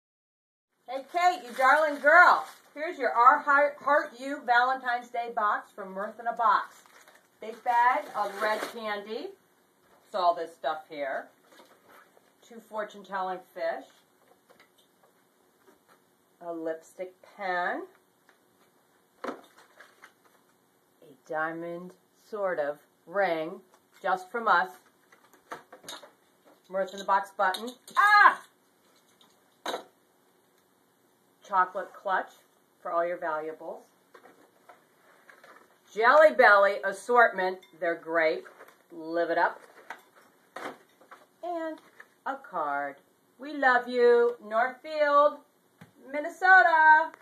Care Package to Carleton College Northfield, Minnesota
Care Package to Carleton College Northfield, Minnesota, Today we are packing a Valentines Box for Kate at Carleton College in Northfield Minnesota.